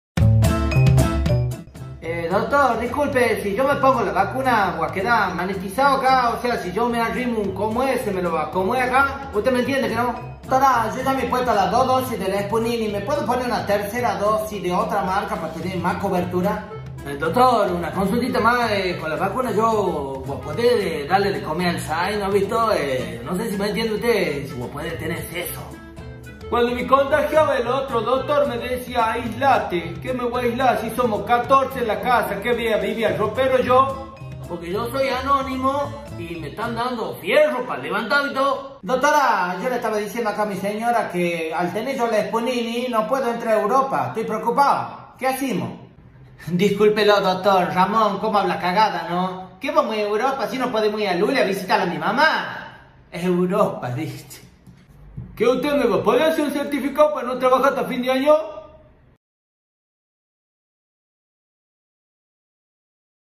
los chicos no se quieren vacunar y molestan al doctor milagro🎅🥂🤶.
agustinosaurus rex.